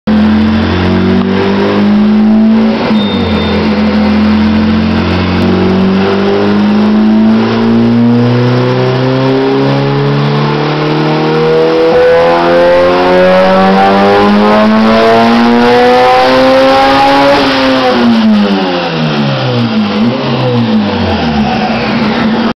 Civic EK turbo dyno run
made 384hp at 19 pounds of boost